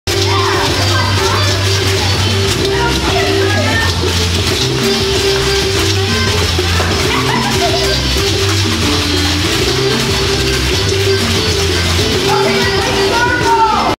Summer Camp shaking it!
Rise summer camp kids create music with homemade shakers